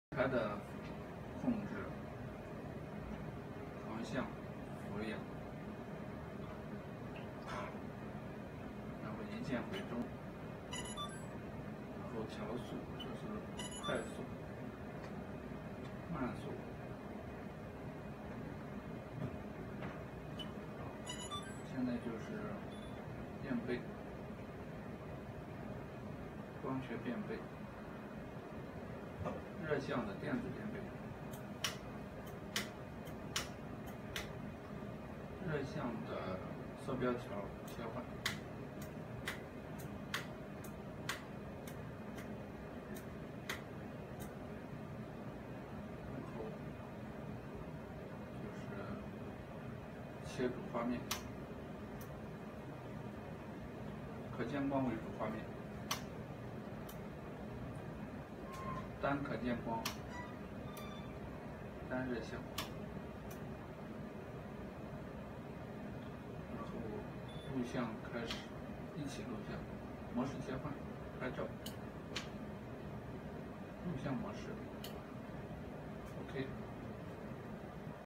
HK GL Dual Zoom and Thermal Image Integrated Camera
Dual Zoomable and Thermal Imaging Integrated Camera for Military Surveillance(640 Flir) 18* and 30* Zoom